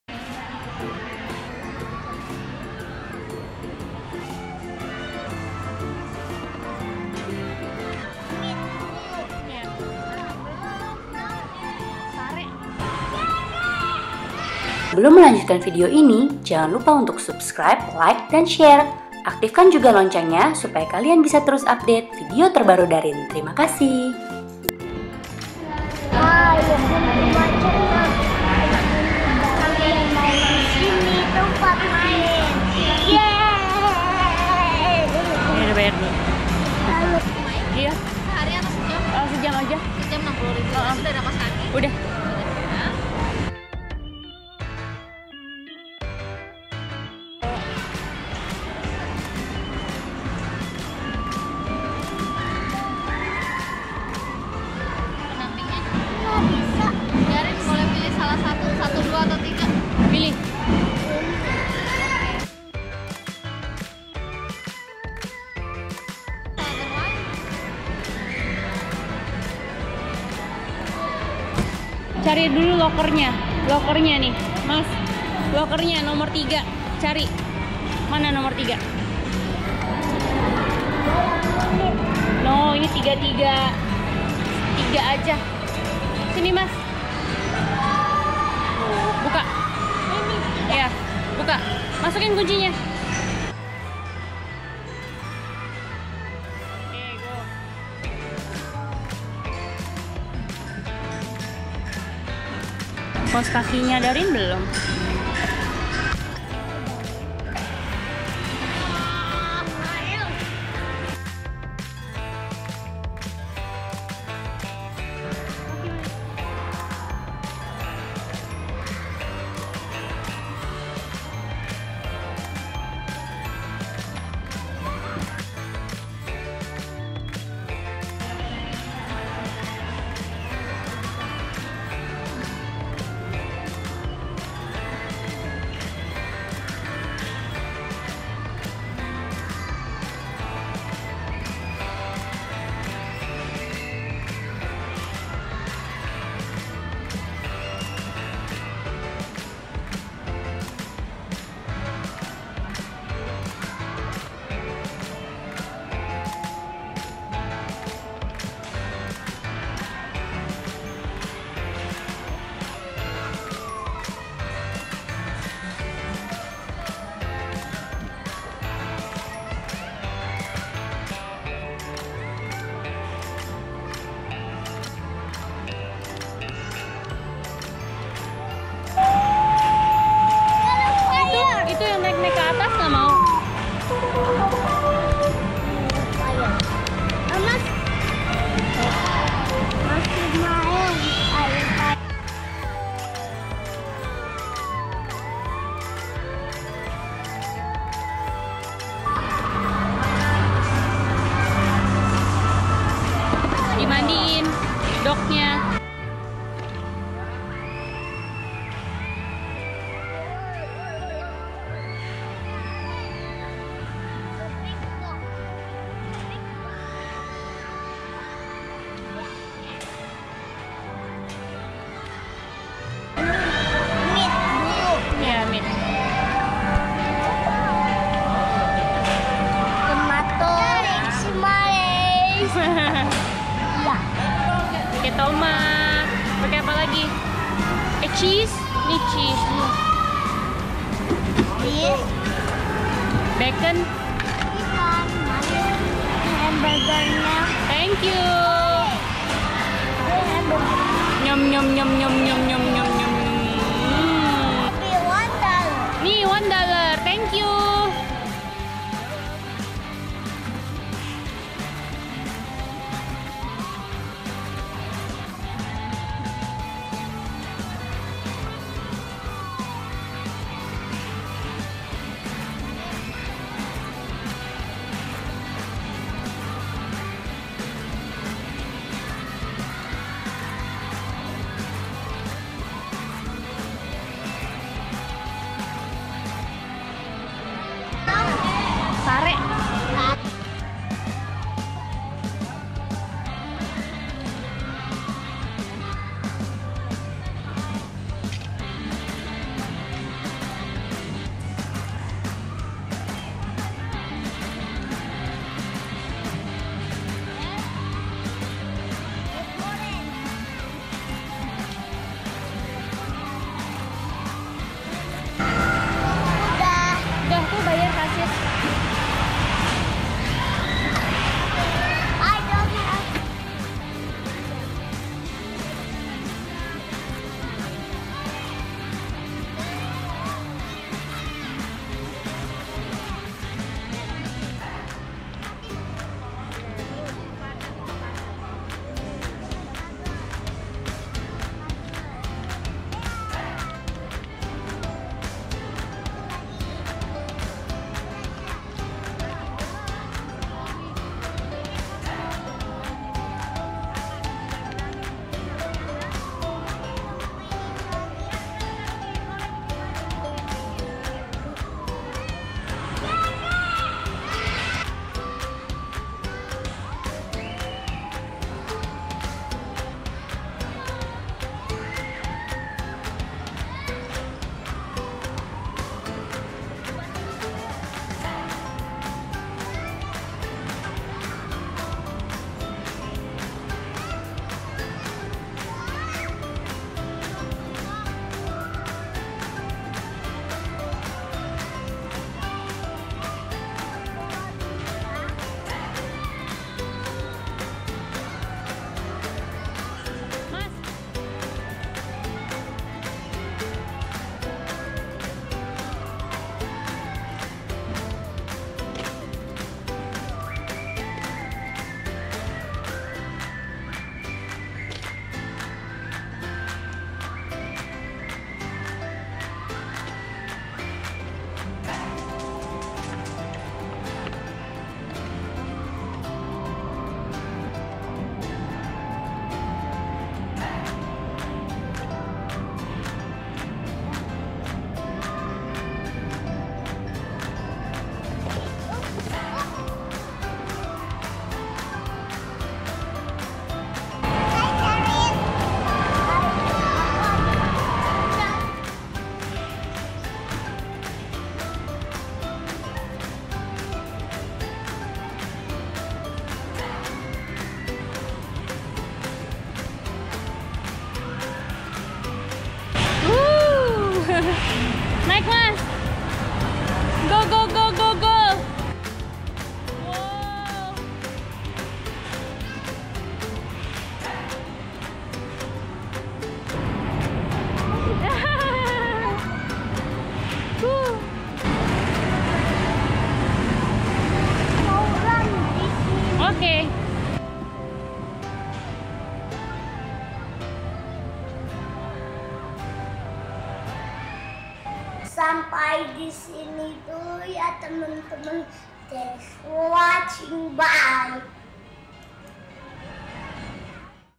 Darin Bermain Di Kidzoona Lotte Shopping Avenue | Pretend Play di Kidzoona Jakarta
Hari ini Darin mau main di Kidzoona Lotte Shopping Avenue Jakarta nih. Tempatnya masih baru loh..

Yuk ikut keseruan Darin..